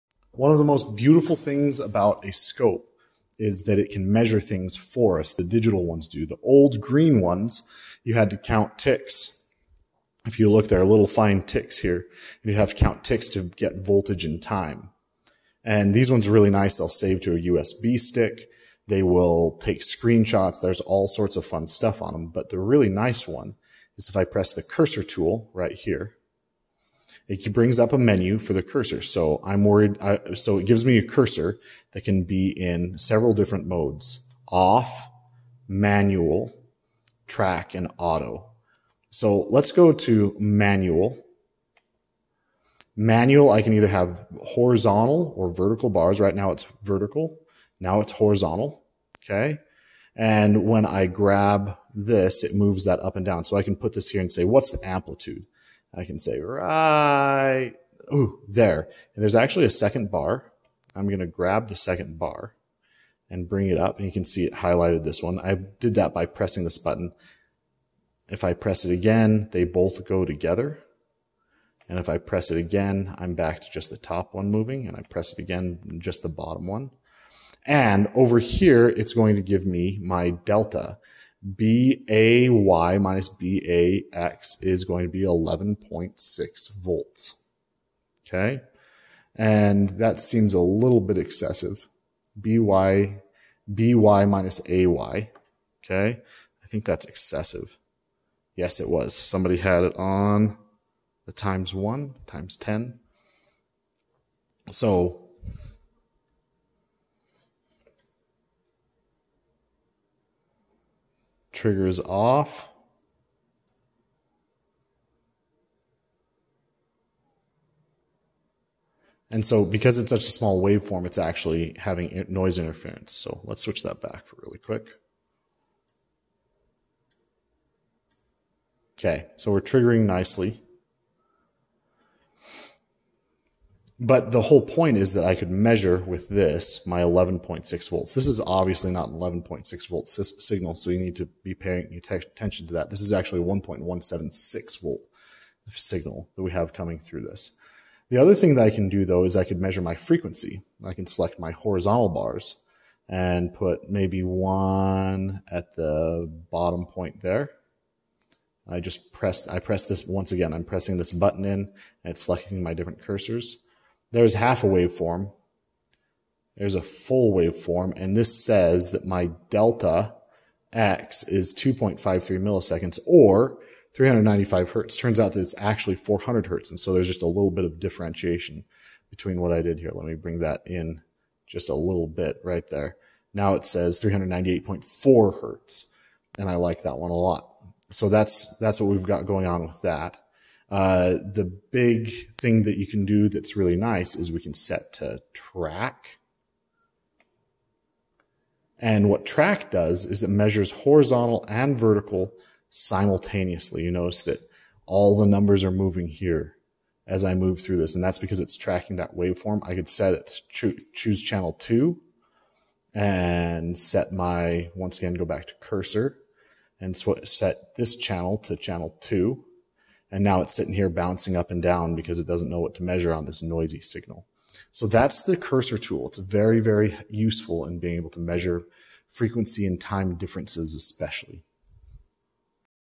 Using the cursor tool on a RIGOL DS1104
For Montana Tech EE Students (Though others may benefit)
How to use cursors to measure voltage and time differences using the RIGOL DS1104 O-Scope